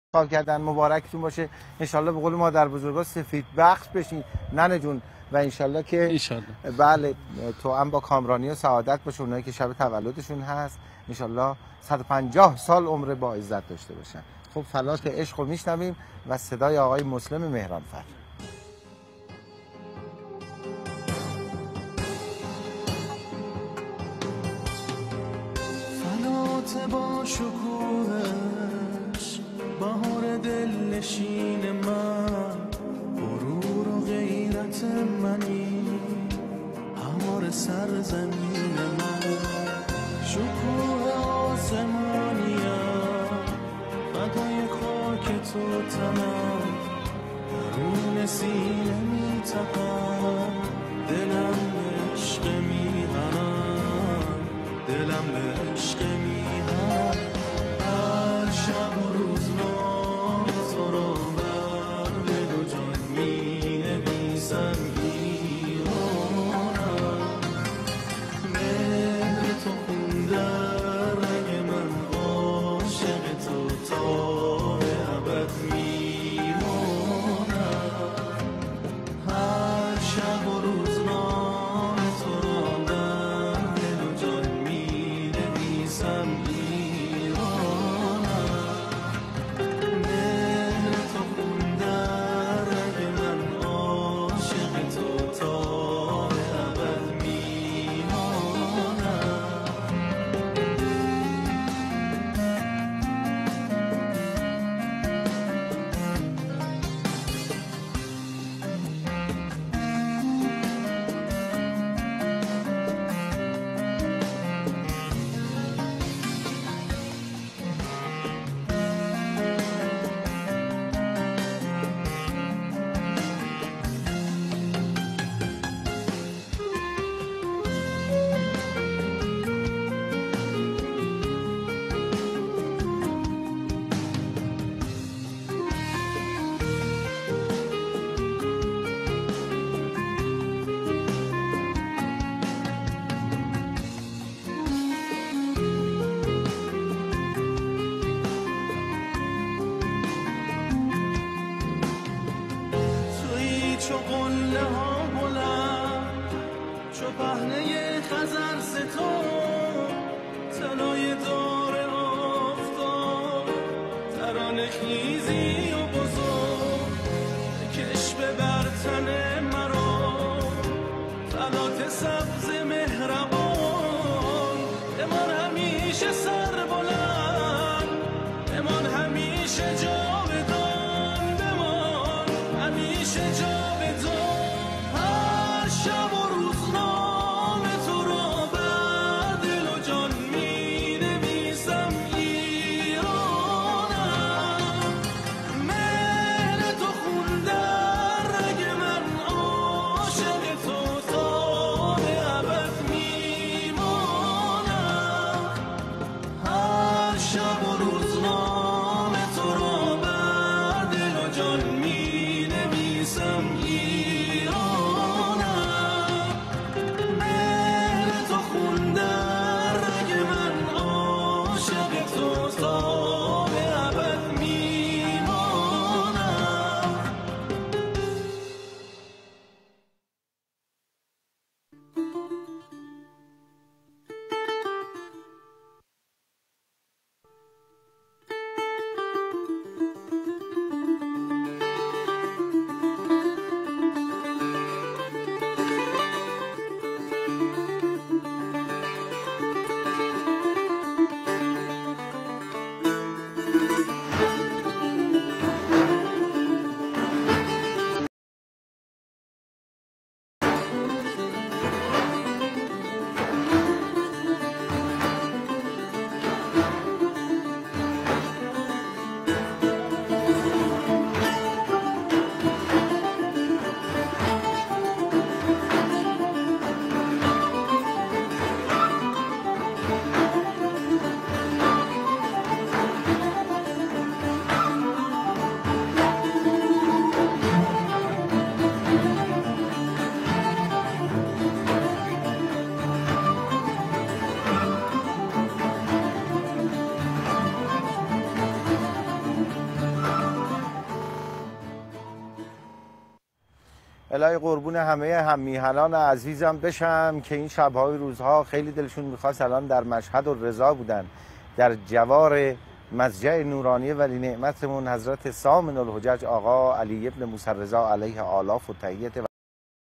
IRIB KHORASAN RAZAVI920321 214932] x264
اولین اجرای زنده همسر عزیزم در شبکه خراسان رضوی